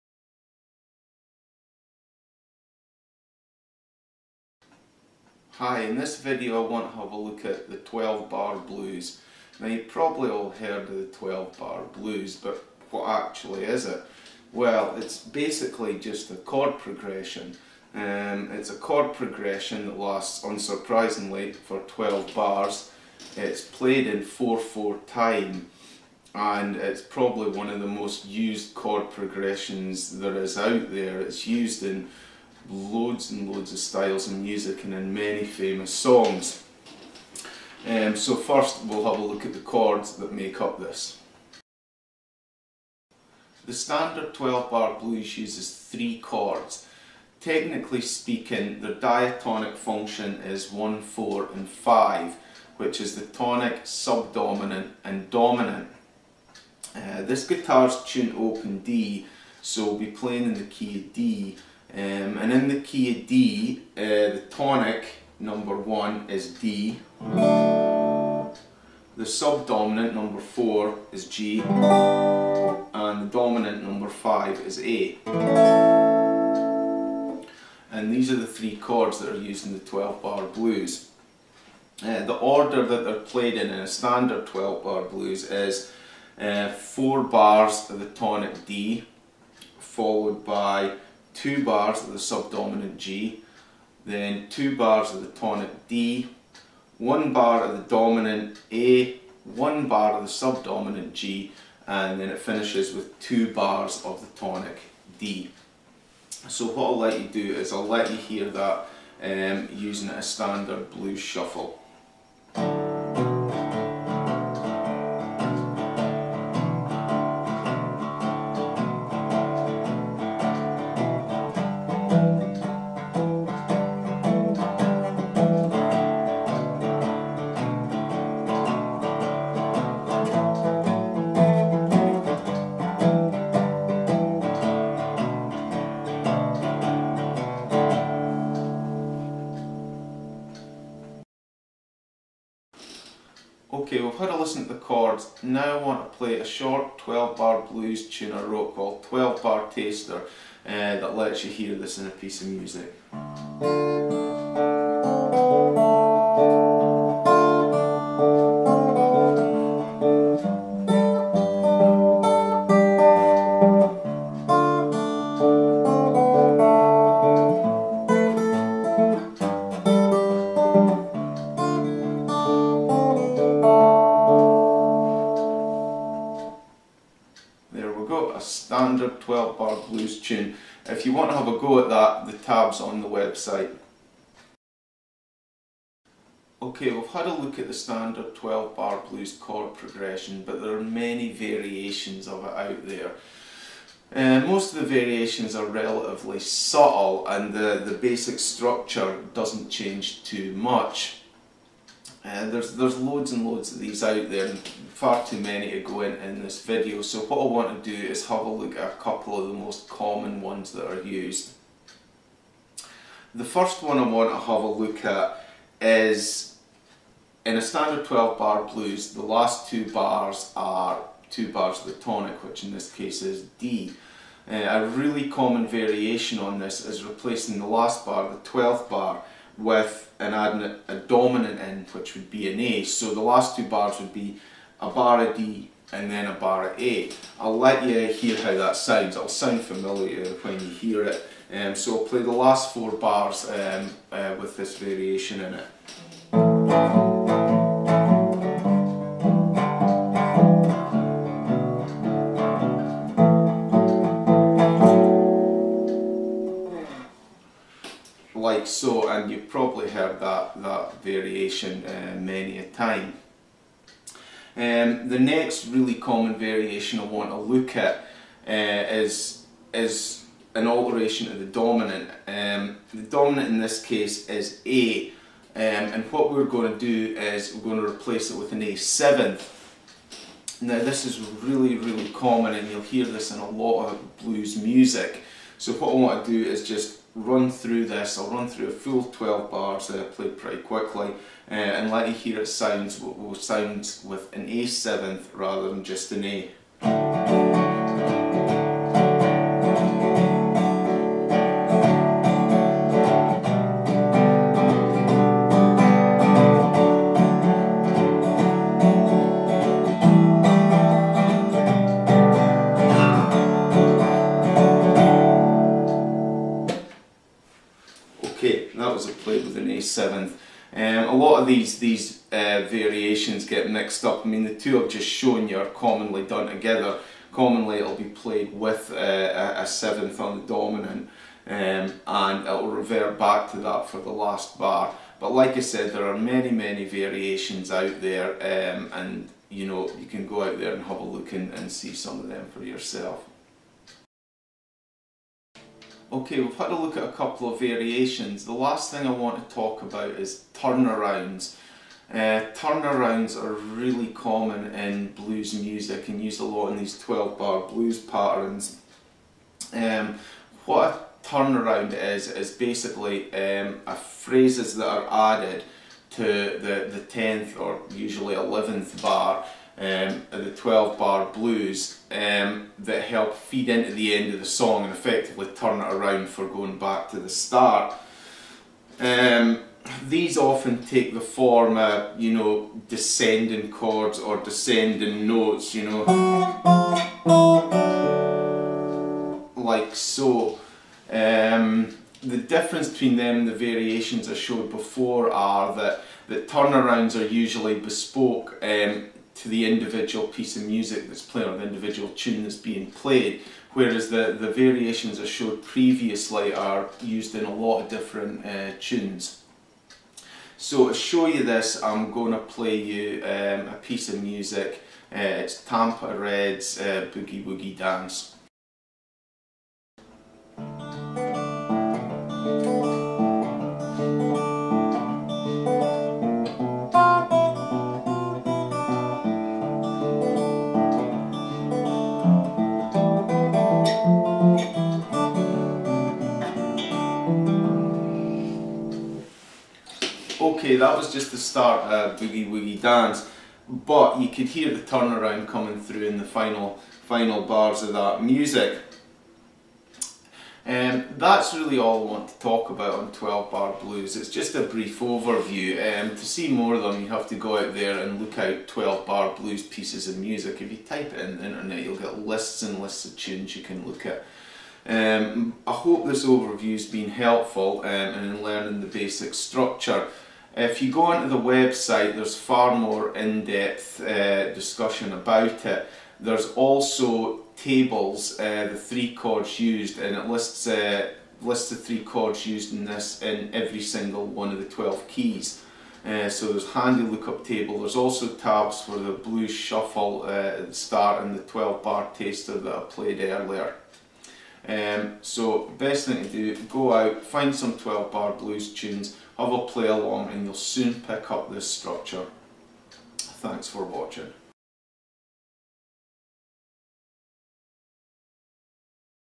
How to Play 12 Bar Blues on Guitar
An overview of the 12 Bar Blues and how to play it on guitar